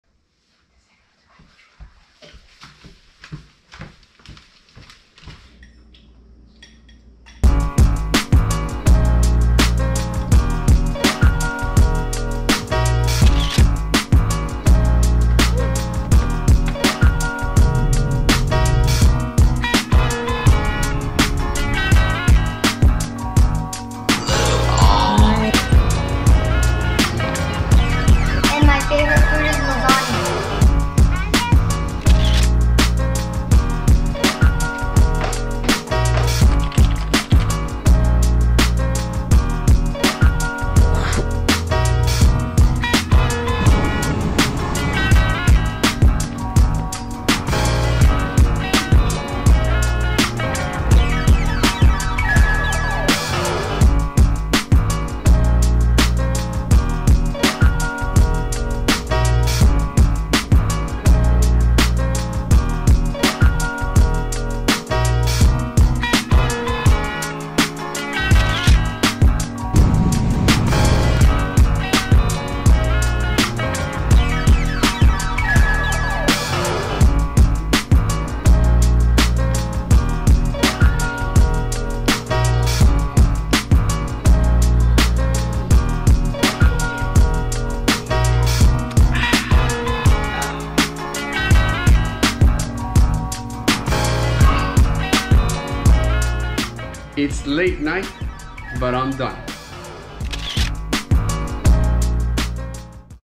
Back to School (Covid-19 edition)
Classes started and just like many, our school district opted for virtual learning. Luckily for our little one, still goes to Pre-K so she can still enjoy a “normal” lifestyle. It’s a change but like everyone, gotta be positive and hope that this ends soon!

Edited on iPad Pro 11” Pro on LumaFusion.